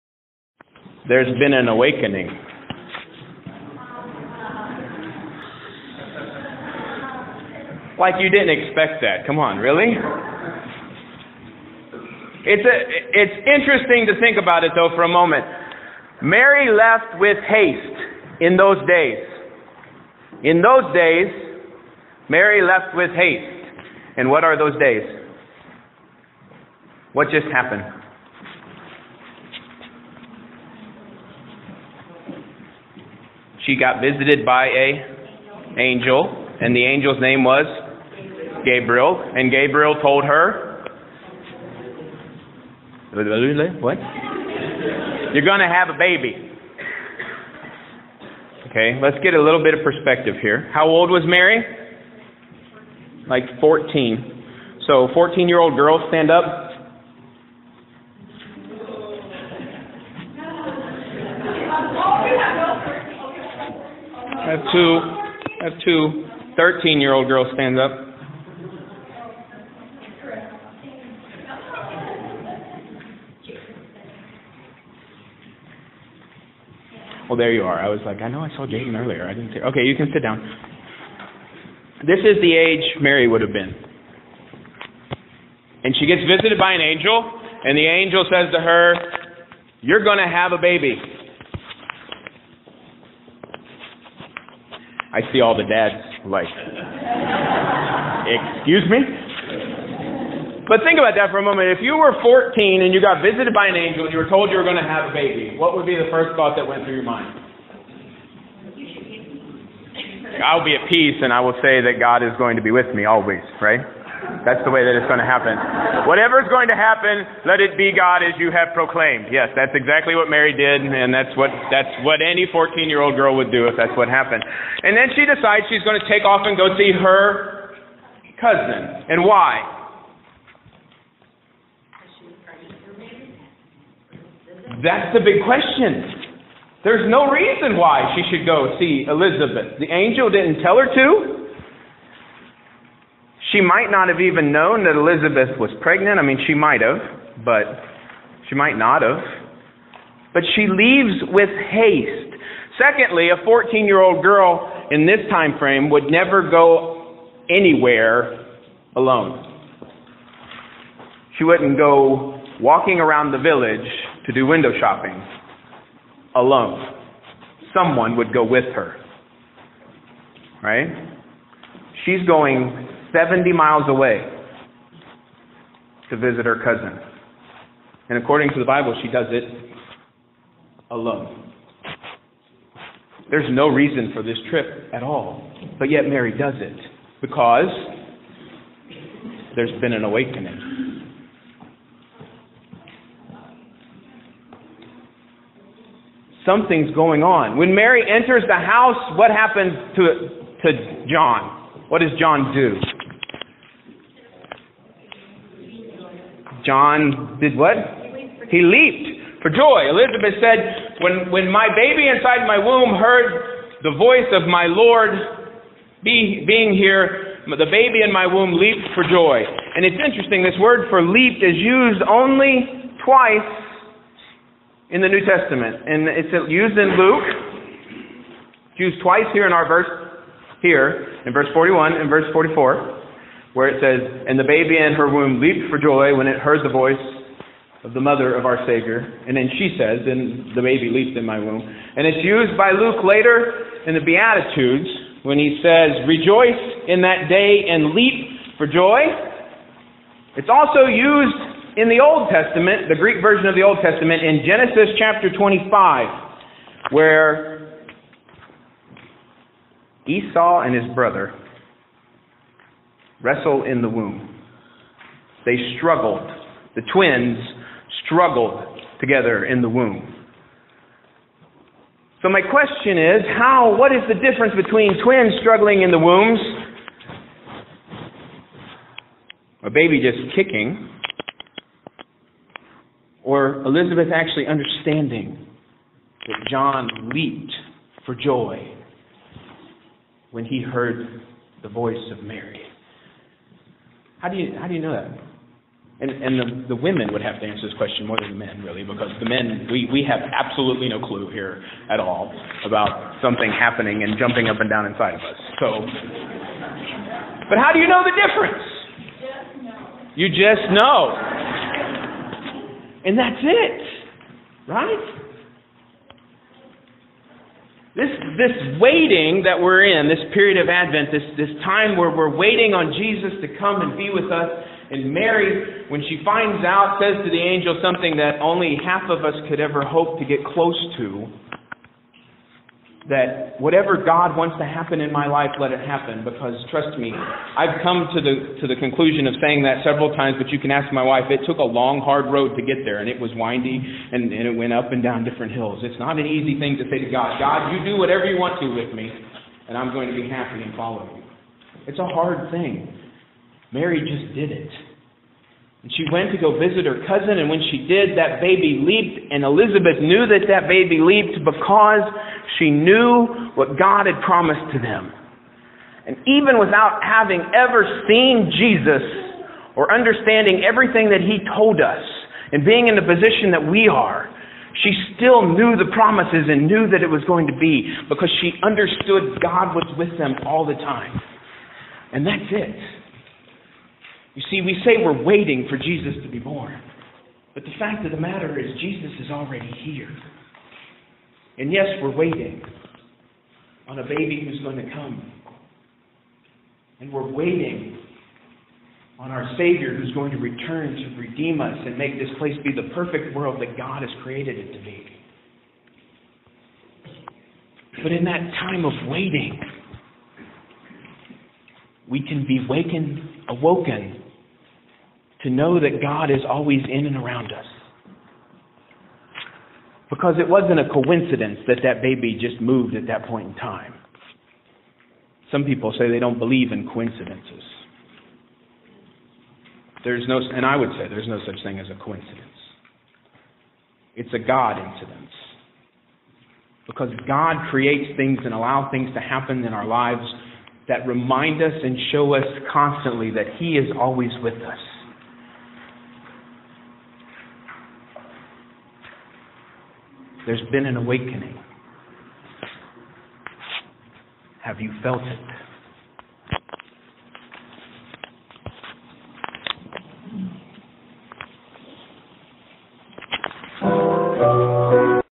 Faith Awakens
Sermon preached at St. John's Lutheran Church in Little Suamico, WI by Pastor Jerry Wirtley on 12/20/2015